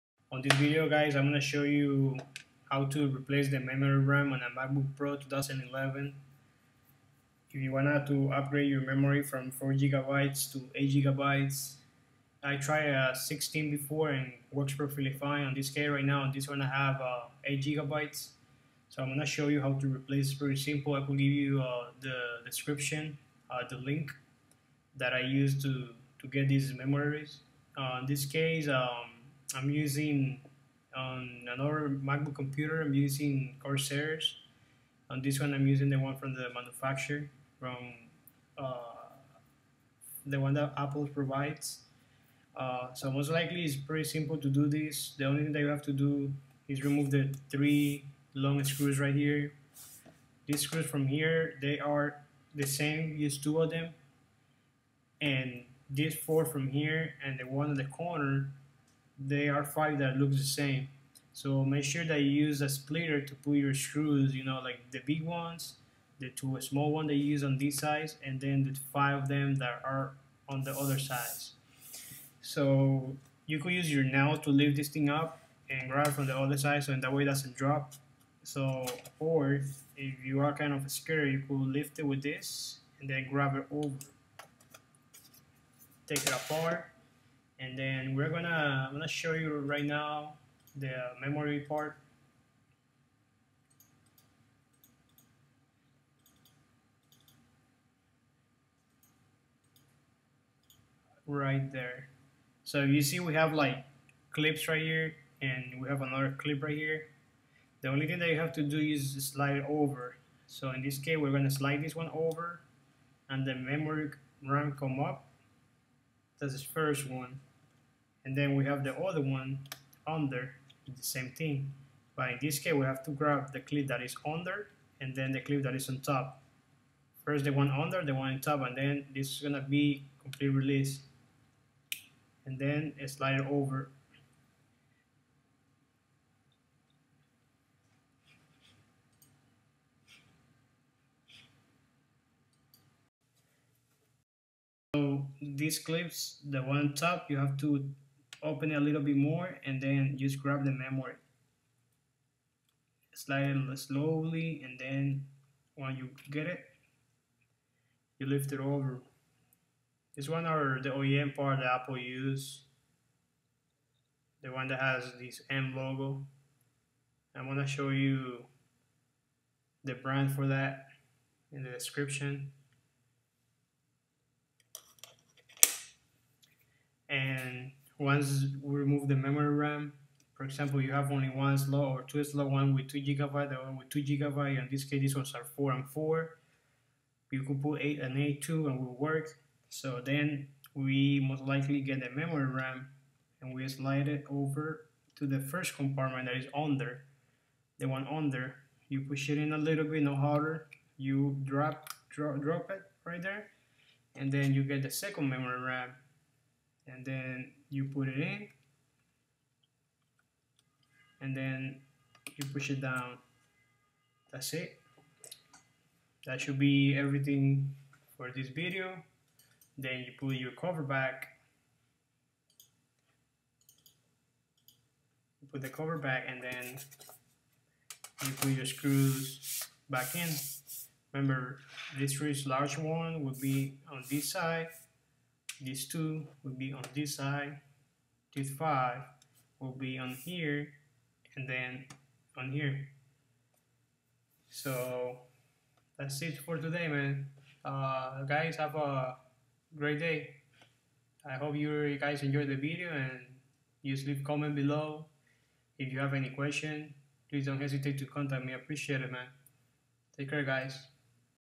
MEMORY RAM UPGRADE MACBOOK PRO 2011 TO 2015 FAT MODEL
How to replace memory ram for Macbook PRO 2011 or if you want to upgraded it from 4GB to 8GB or from 4GB to 16 GB. the maximum memory ram that I tried has been 16GB they worked fine.

I got this ones and I used them over 3 years without not problem, then I decided to use them in a different computer. See links:

8GB MEMORY RAM SET
16GB MEMORY RAM SET

If you want to restore your MacBook Pro 2011 without any dvd or usb flash . With a brand new hard drive.